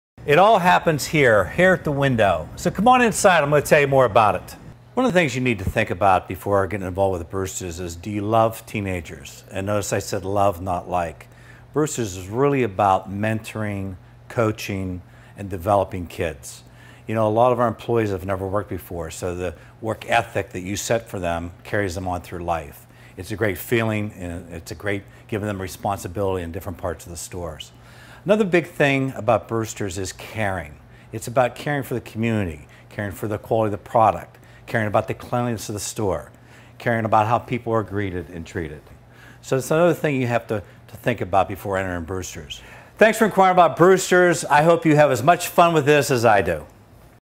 Bruster's Ice Cream
Thinking about owning a Bruster's? Here's a message from our founder, Bruce Reed. The "Bruce" in Bruster's.